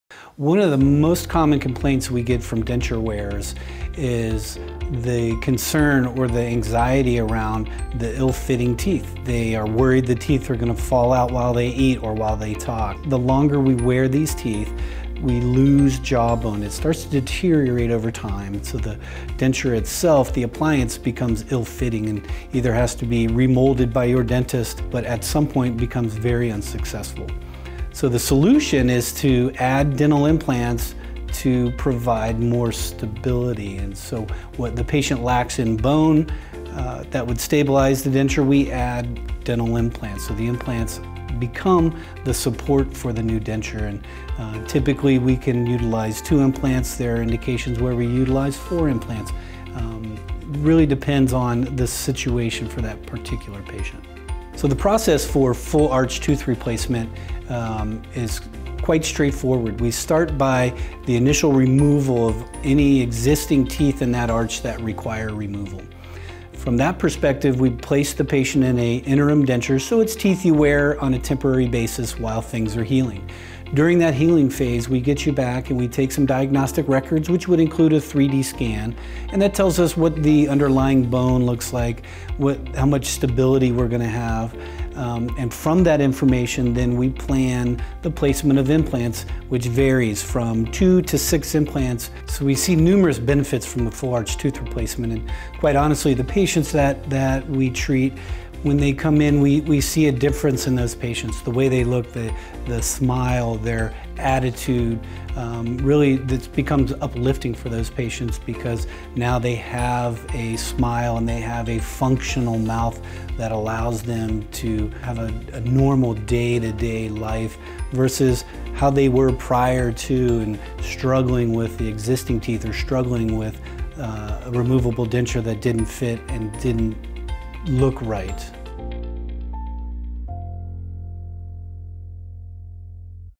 Dentures to implants - full arch options
Dr. Ellis talks about full arch options. Denture wearers always exclaim that they are embarrassed of their teeth slipping and the dentures becoming ill fitting. The solution is to add dental implants to provide stability. This allows for the jaw bone to stay secure and dense, and no worrying about dentures being faulty in day to day activities. Ellis Oral Surgery offers many dental implant options for their patients, so everyone can enjoy permanent and durable teeth with a flawless smile.

Ellis Oral Surgery

Gahanna:
420 Beecher Road, Suite A
Gahanna, Ohio 43230

Upper Arlington:
760 Communications Parkway, Suite 100
Columbus, Ohio 43214